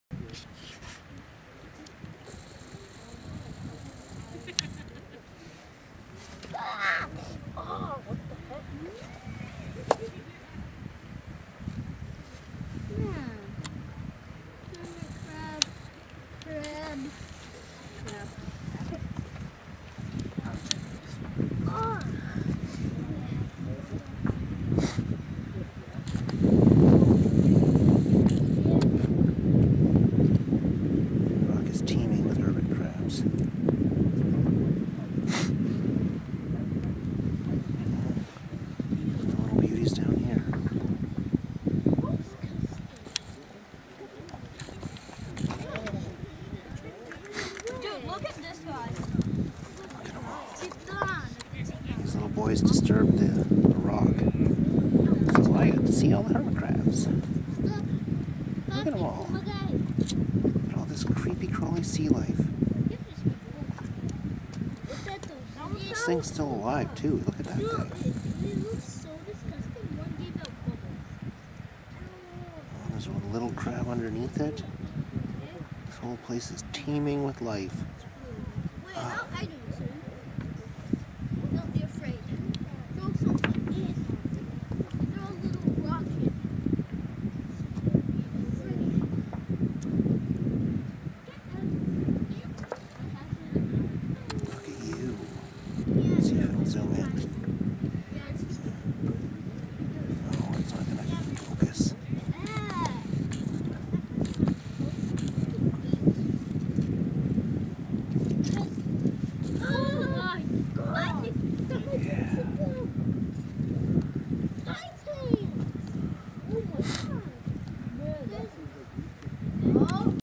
Tons of Cutie Crabs
I found this mass of hermit crabs and other assorted crustaceans after some young boys turned over some beach rocks in the tidal flats during low tide.  You can see tons of hermit crabs, as well as a baby dungeoness crab as well as a baby puget sound king crab.